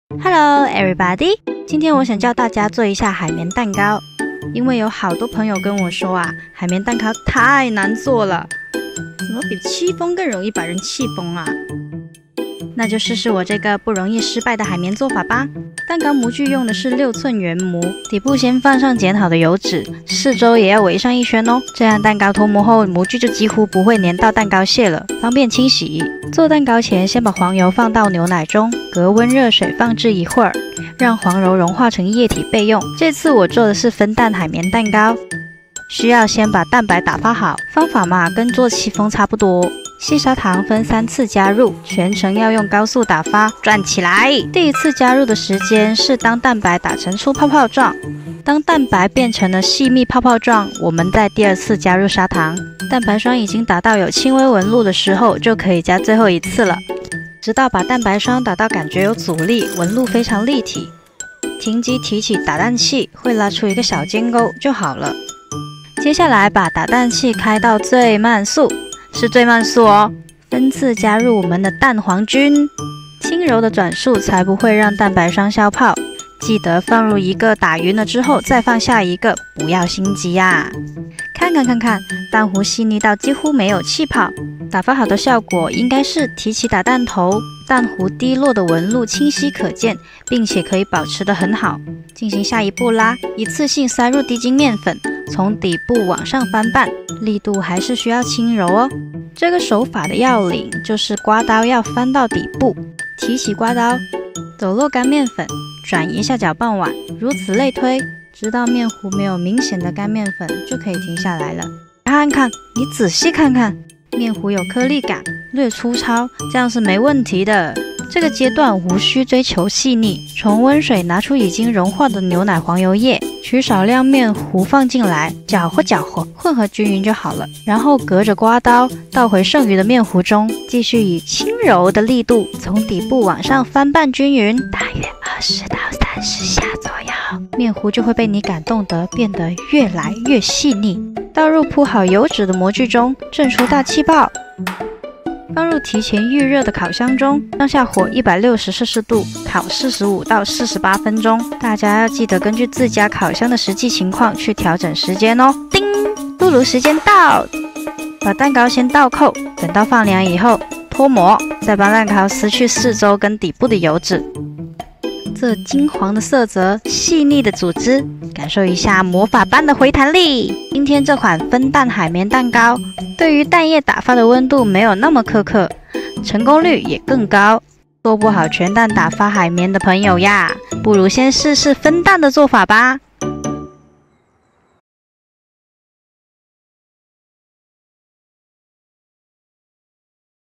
【不藏私】零失败的海绵蛋糕 | the secret of a perfect spong cake
原来有不少朋友一直做不好海绵蛋糕，直言它比戚风更容易让人“气疯”😂一个不小心，制作过程就会发生消泡，最后烤成扁扁的蛋饼。实不相瞒，戚风蛋糕的前身就是海绵蛋糕。海绵蛋糕的制作方式有两种，分为分蛋法以及全蛋法。

戚风用了分蛋打发的方式制作，配方中的液体比例更加高，所以它的质地会来得更蓬松、柔软湿润，很适合直接食用。也因为这样极致的柔软，戚风胚的夹馅承重力、弹性的口感，不够海绵蛋糕好。很多日韩甜点师，会考虑食材组合之间，不同的口感搭配，优先选择海绵蛋糕作为基底。

海绵蛋糕的制作方法中，充满着制作糕点的技巧和基础，特别是打发鸡蛋的方法、混拌面糊的手法，都是非常重要的，是“制胜”关键。对于做不好海绵蛋糕的朋友，我推荐你们从下面视频的分蛋法制作入手。在打发鸡蛋这步上，它比全蛋法更容易让人掌握，不容易失败。

🟤食材 //ingredients
蛋白  eggwhite  105g
蛋黄 eggyork 54g
低筋面粉  cakeflour 80g
细砂糖 65g sugar 65g
牛奶 milk  35-40g
黄油 butter 25g

🟤细节补充//Tips：
1、全蛋法在全蛋中加入砂糖，需要隔水加热至35-40℃之间，这样才能比较容易打发出气泡。分蛋法则对于温度要求没那么敏感，直接从冰箱拿出鸡蛋，分出蛋白蛋黄后就能制作了，蛋白无需再冷冻。

2、很多人做点心都想减少糖的用量，但海绵蛋糕的糖量不要轻易减少，否则会让蛋糕不易膨胀，保水性变差，吃着很干、松散，不柔软，也不耐放。

3、操作中哪几个方面，能避免消泡？
① 当把蛋白霜打成小尖钩后，分次加入蛋黄时，手持电动打蛋器需要用最低速来拌匀。这样才能形成气泡非常小、细致均匀的蛋糊。气泡越小，接下来加入低粉、黄油牛奶液的操作中，越不容易消泡。
②黄油牛奶液隔温热水或者微波炉加热熔化，熔化后的液体温度在60℃左右比较好。温度太低，难以平均分散于面糊中，增加搅拌次数会使蛋糊气泡被破坏（逐渐消泡）；温度太高，则让蛋糊气泡的表面张力变差，蛋糕的口感变得粗糙。
③先让少量的面糊与熔化黄油牛奶液拌匀，是因为液体与面糊的比重、质感密度有很大差别，不容易拌匀。先取少量与另一种材料拌匀，再加入剩余的材料，就比较容易完成，不会因为多次翻拌而导致消泡。
④隔着刮刀把黄油牛奶液浇淋在面糊上，能让液体更均匀地分布，减少翻拌次数。